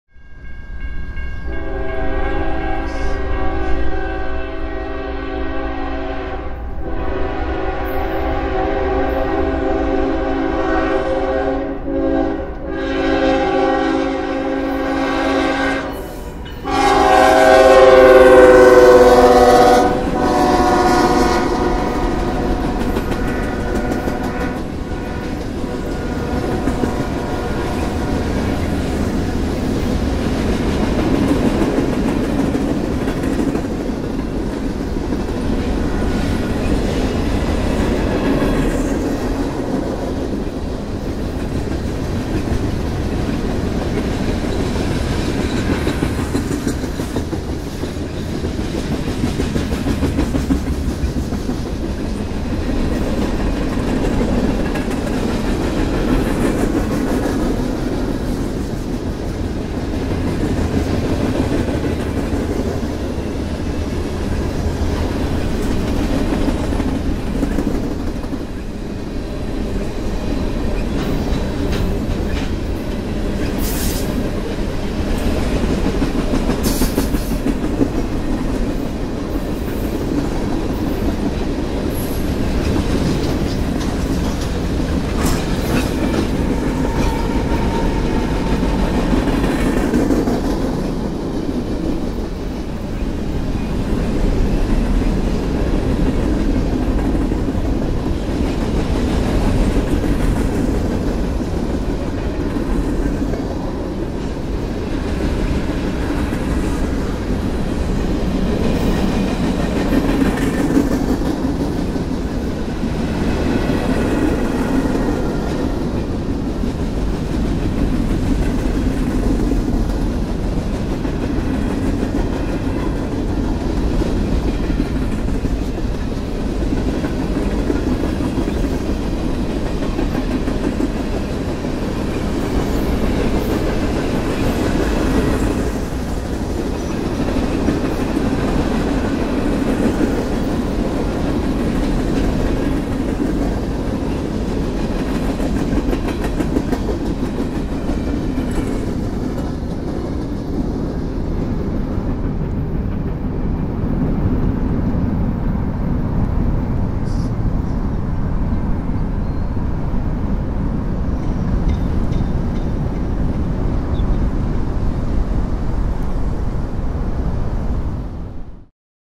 Single Locomotive Up Front on CSX 521
CSX 521 is southbound at Pryor Street in Athens, Alabama near Pryor Street. The building with the two story section to the right in the background was built as the L&N freight depot, now a military museum.

The defect detector gave the axle count as an even 500.

1:19 - Distributed Power

The locomotives are CSX 3150 leading and CSX 893 in the middle.